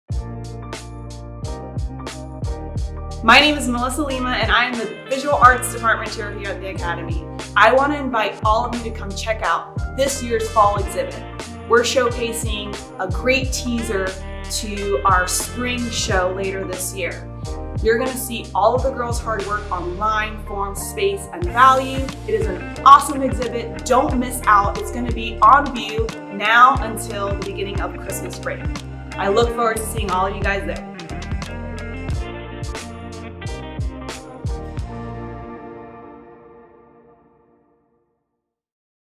AHN Spring Art Exhibit 2022
The High School Fall Visual Arts Exhibit “Collective Lineation” is now open in the Michael & Candace Valdes Gallery through January 1, 2023. Check out the artistic talents of our students featuring darkroom photography, drawing, 2D and 3D honors, and sculpture!